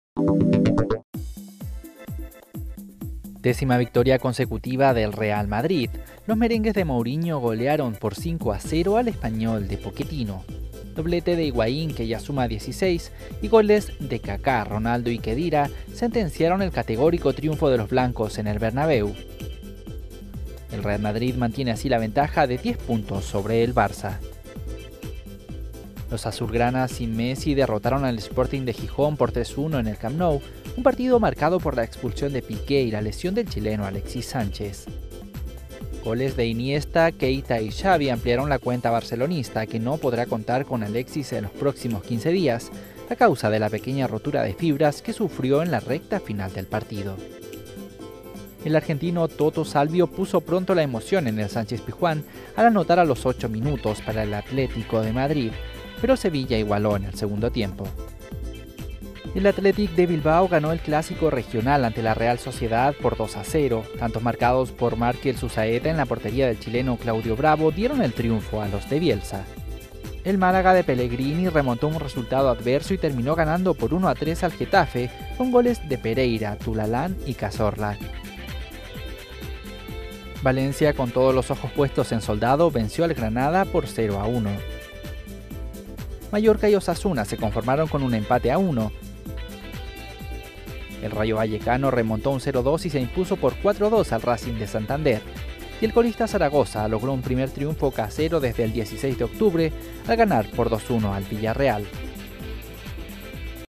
Higuaín lidera la goleada del Real Madrid
El Real Madrid logró su décimo triunfo seguido y echó otro cerrojo de seguridad a sus aspiraciones de conquistar el título, en una jornada en la que tampoco falló el Barcelona, su inmediato perseguidor a 10 puntos de distancia. (Video EFE)